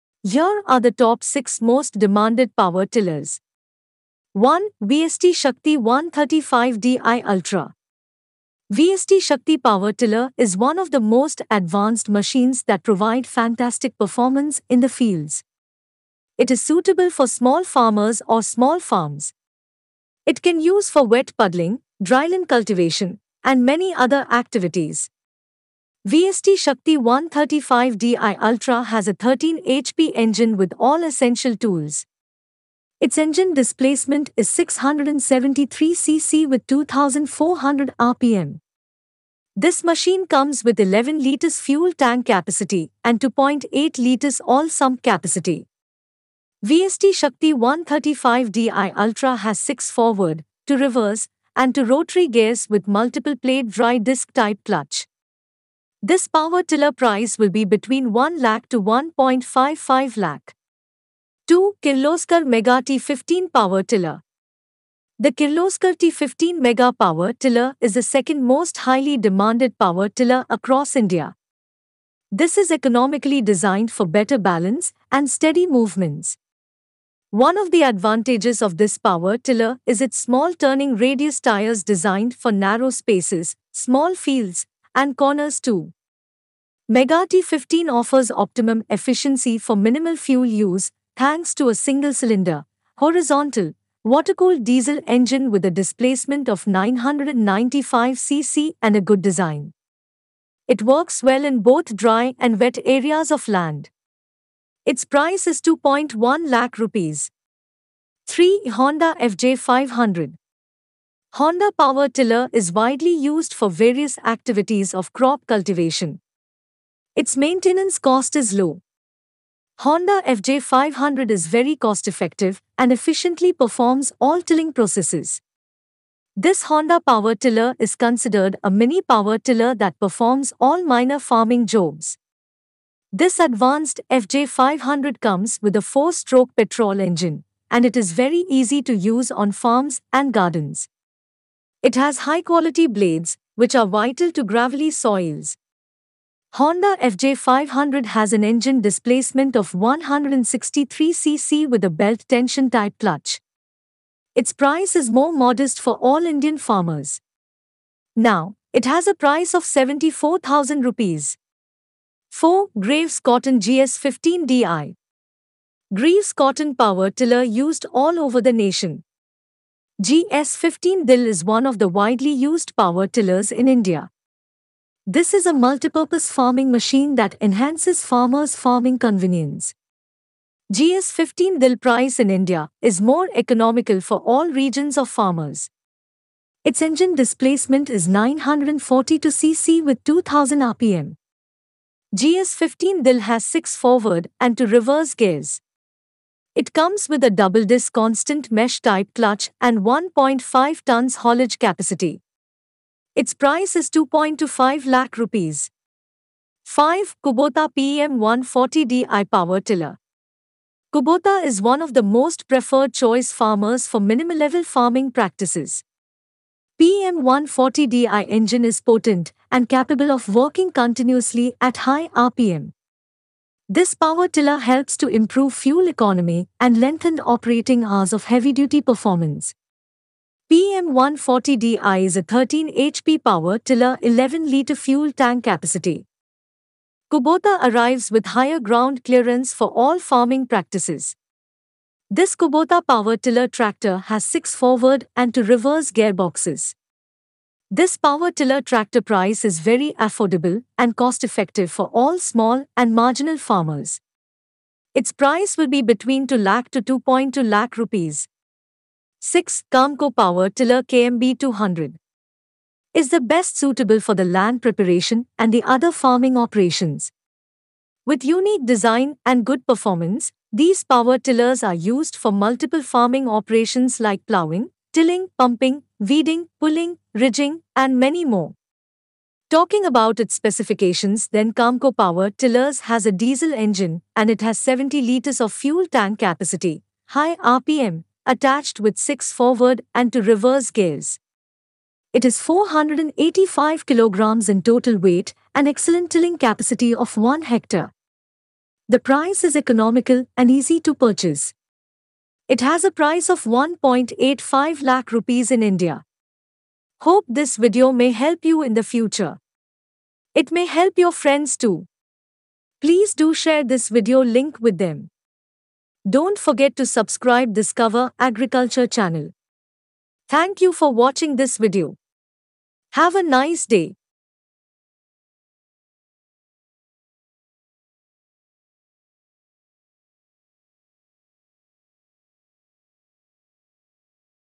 Best Power tiller Machines (Price & Specifications) | Top 6 Hand tractor Cultivators
The power Tiller is one of the great inventions in the agricultural field. It is the most useful and valuable farming tool to make farming work more comfortable and relaxing. Several minor and advanced activities were performed with various tools in farming, and a power tiller was mostly used for minor agricultural activities.

The power tiller is a multipurpose hand tractor intended primarily for rotary tilling and other small farming operations. It is a hand tractor, garden tractor, walking tractor, or two-wheel tractor. The cultivators initially included rotavator attachments, trailers and plows, and riders with power tillers. It favored small landholding farmers for all agricultural operations. The machine provides possibilities for self-employment in rural areas.

0:00 Best Power tiller Machines
0:04 1) VST Shakti 135 DI Ultra
1:01 2) Kirloskar Mega T 15 Power tiller
1:48 3) Honda FJ 500
2:43 4) Graves Cotton GS 15 DI
3:27 5) Kubota PEM 140 DI power Tiller
4:19 6) Kamco Power Tiller KMB 200
5:07 Bottom line